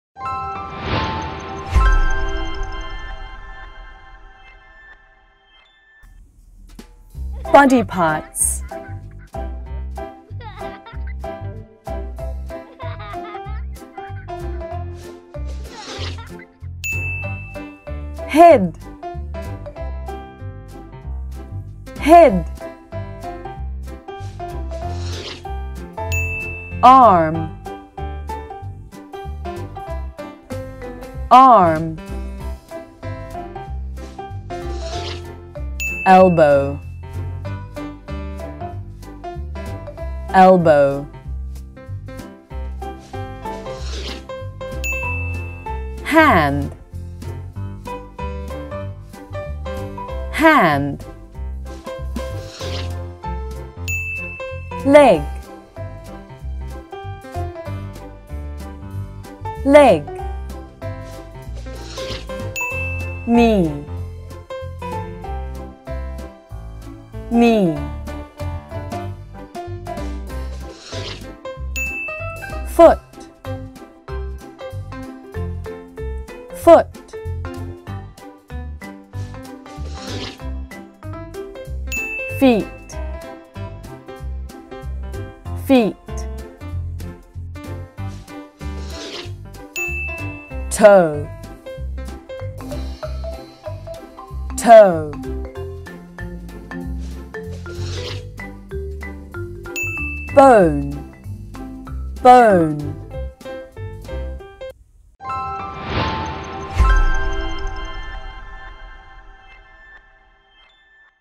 Bé học tiếng Anh chủ đề Các bộ phận cơ thể| Khóa học tiếng Anh cho trẻ em
BingGo Leaders  - Giúp bé học tiếng Anh nhanh & thông minh hơn.